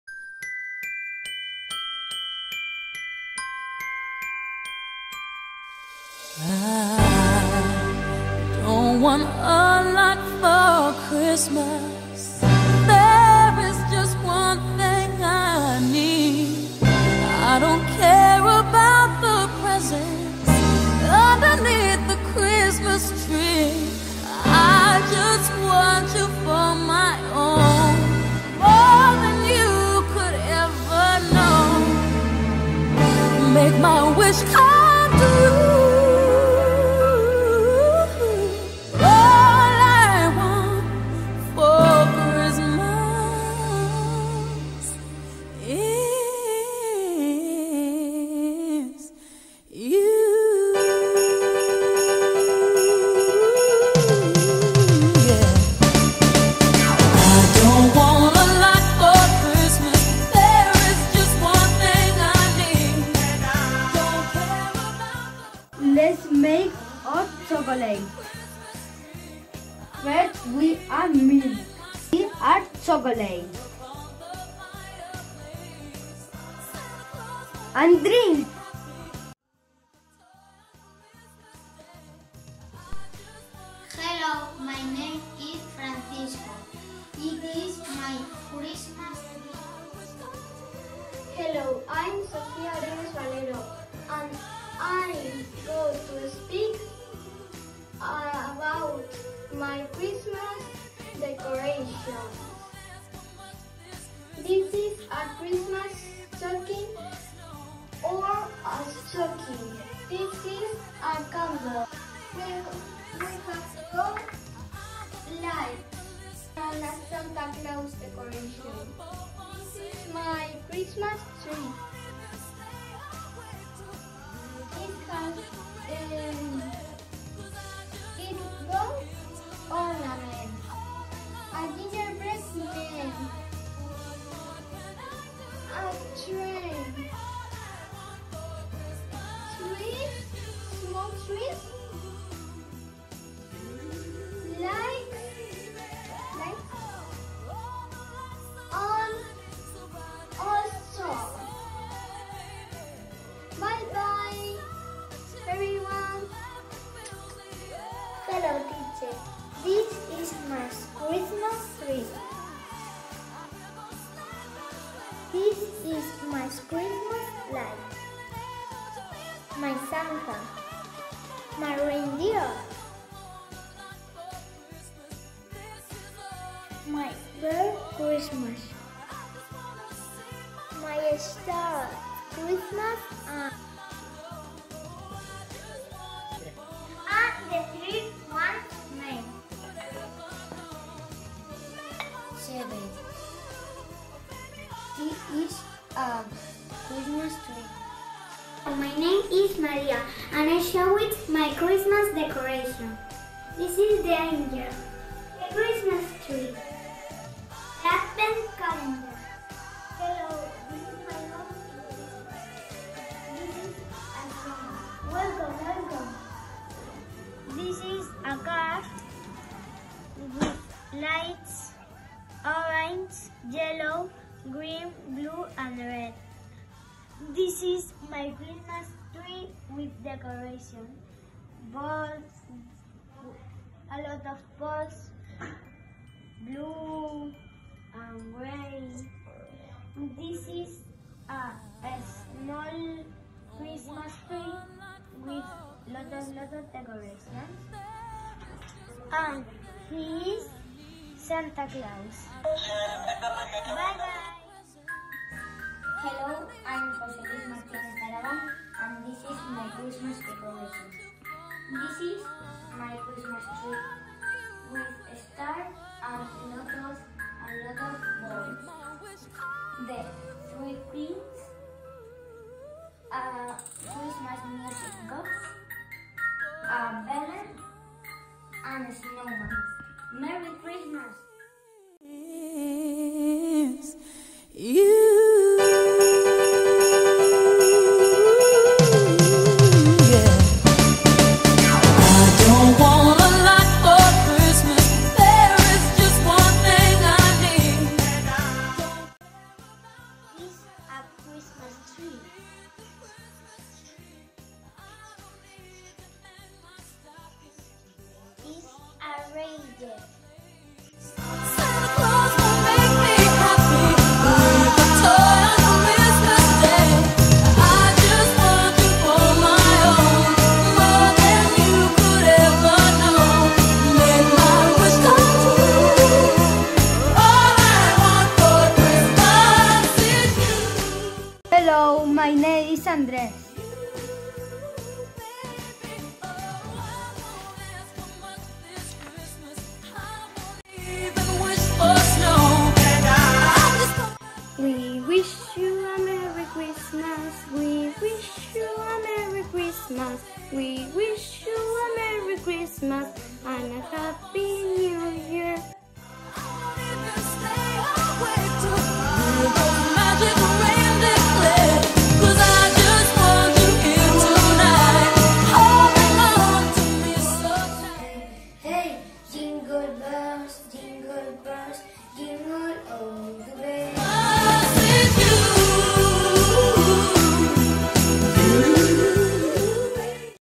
CHRISTMAS CHALLENGES. CEIP San Fernando (Lorca)
Los niños y niñas de 4° han participado esta Navidad en algunos retos navideños voluntarios con el fin de usar la lengua inglesa en escenarios de aprendizaje lúdicos.

Algunos de los retos propuestos han sido realizar tarjetas navideñas o cocinar recetas típicas siguiendo las instrucciones en inglés; crear un vídeo describiendo oralmente la decoración navideña o cantar villancicos en dicha lengua.

Vuestra Teacher Natalia os felicita por el buen trabajo realizado.

Congratulations!